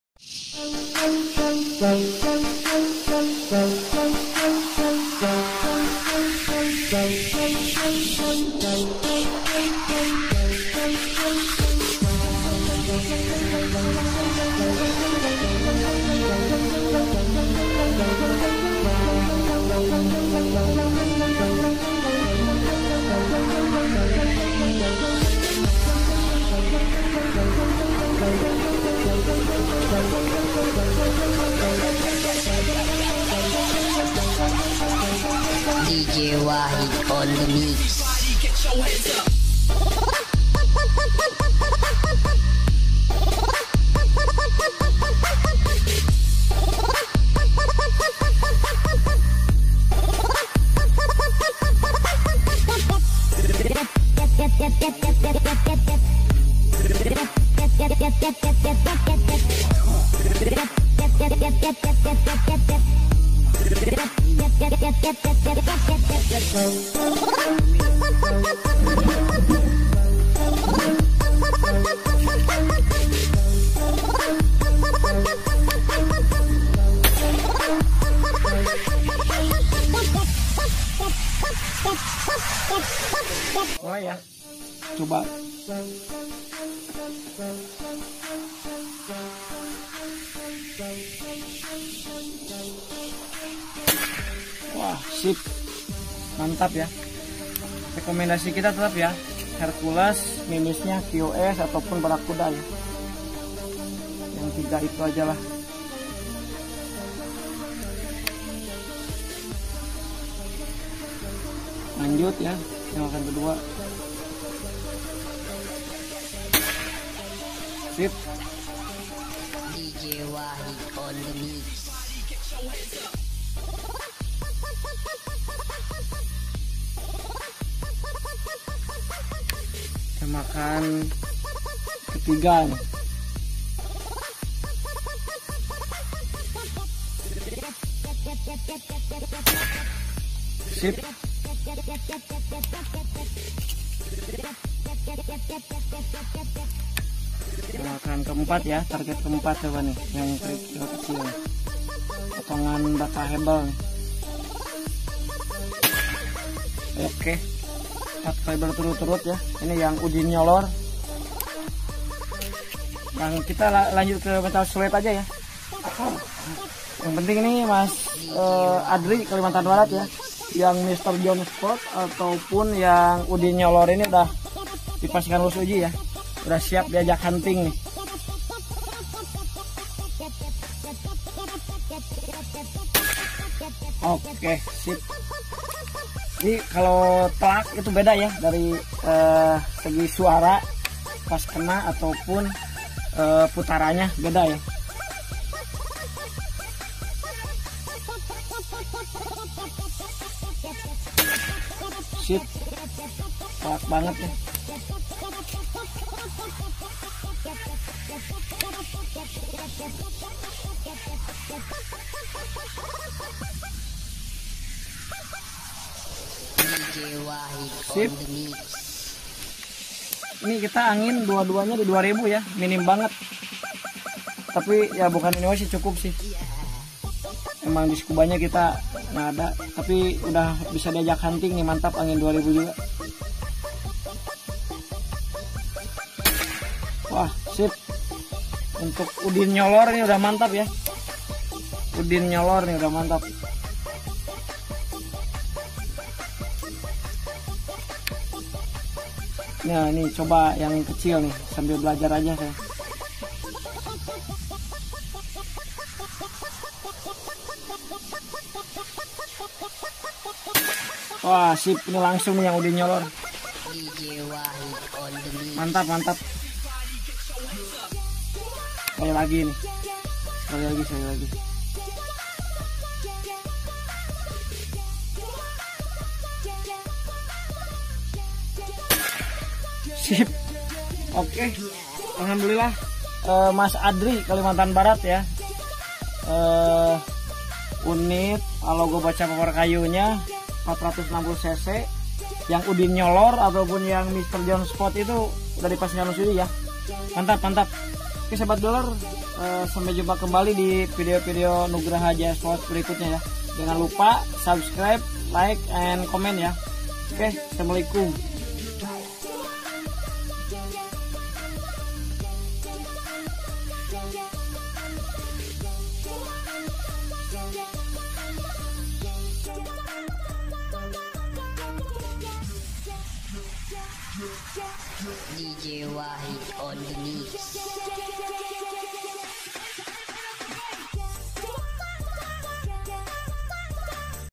AKURASI TERJAMIN!! ALOGO AR BOCAP 460CC POPOR KAYU PUNYA MAS ADRY KALIMANTAN BARAT
Untuk spesifikasi unit:
- Jenis pcp bocap popor kayu
- Tabung 460cc od 50mm
- Laras panjang 60cm
- Serobong bahan Dural od 22 mm
- Box chamber monolite
- Grafir nama di chamber  Udin Nyolor
- Proses pengerjaan box chamber menggunakan mesin cnc router
- Cancel kokang sidelever
- Manometer bawah
- Pengisian angin minikupler
- Model popor kayu sesuai pesanan

Untuk info dan pemesanan hubungi Nugraha Jaya Sport di no tlp/wa.082216341030